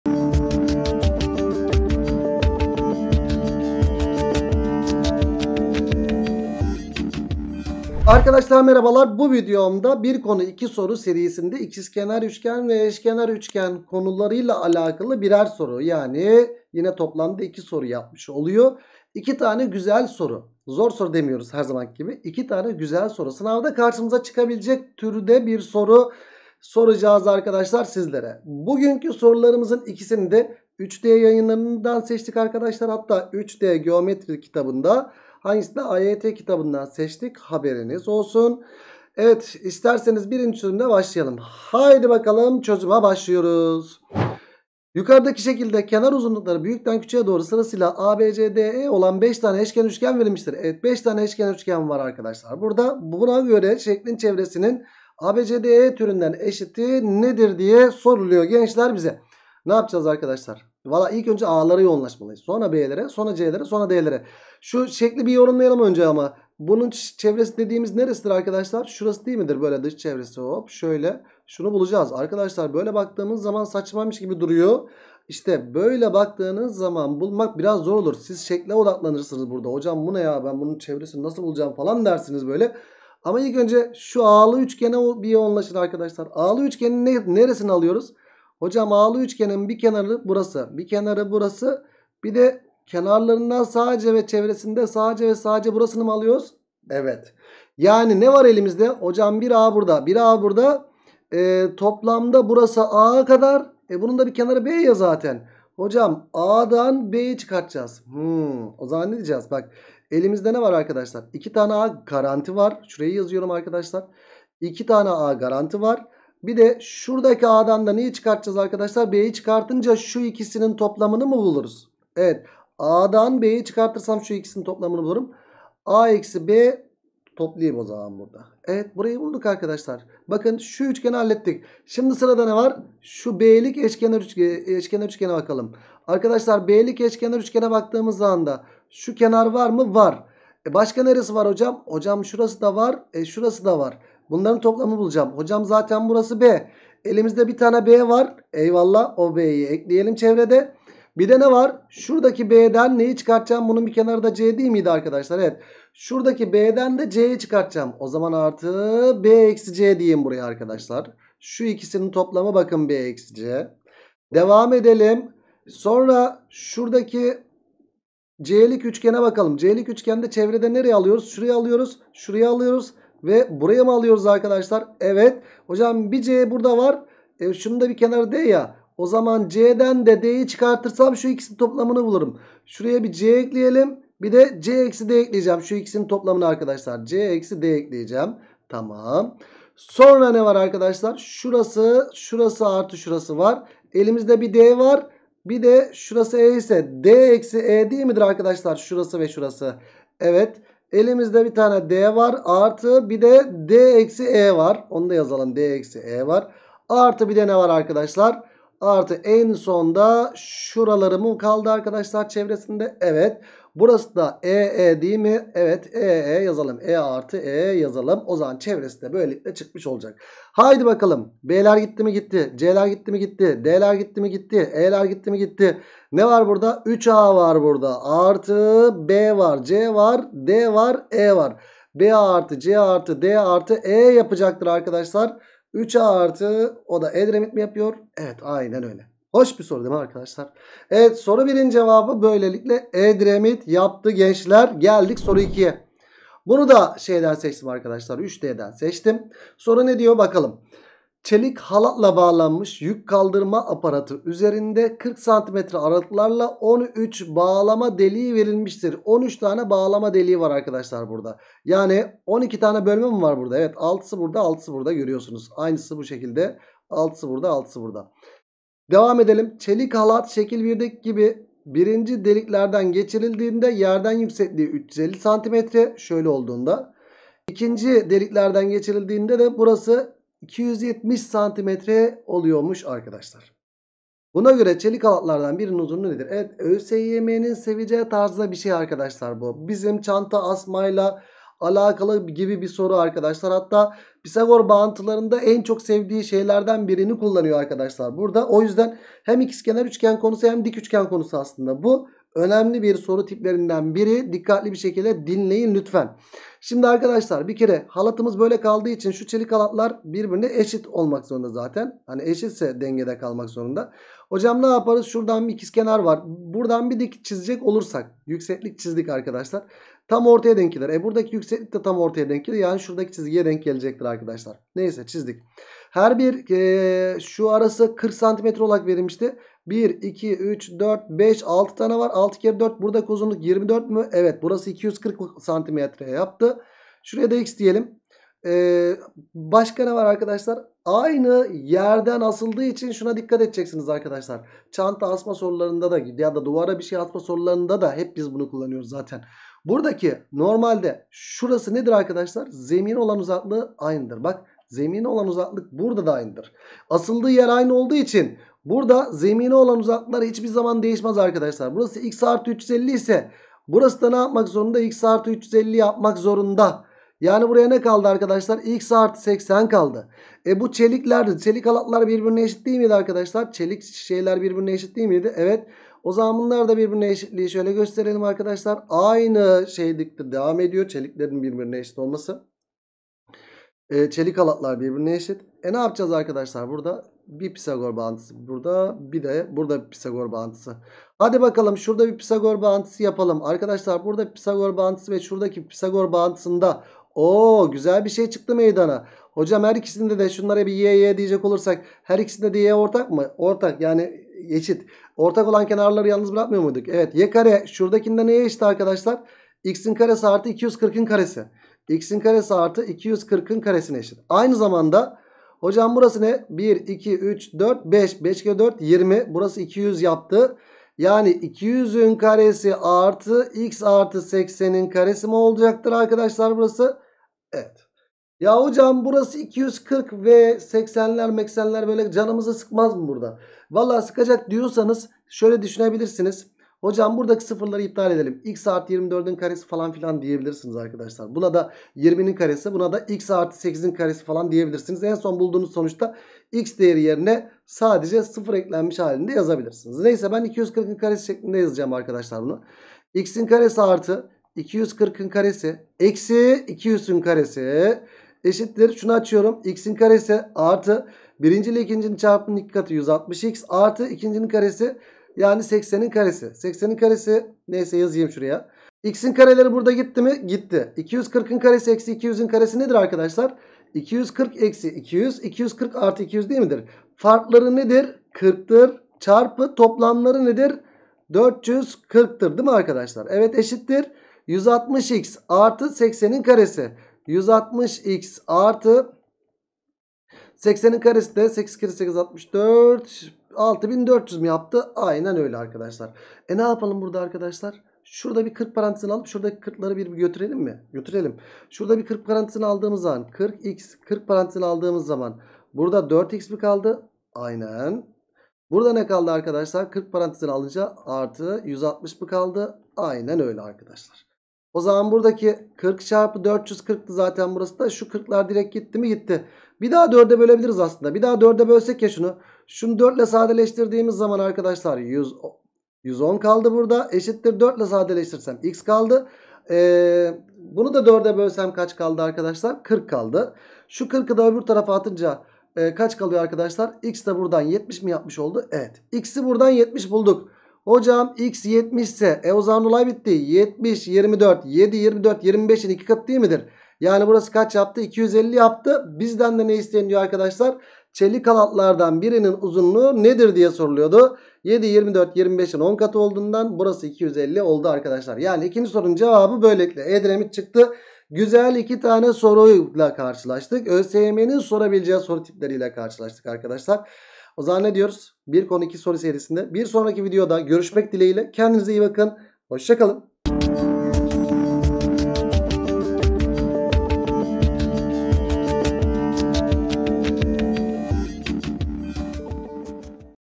1 Konu 2 Soru | İKİZKENAR EŞKENAR
İkizkenar üçgen ve Eşkenar üçgen konusunda YKS sınavında çıkabilecek 2 soru tipi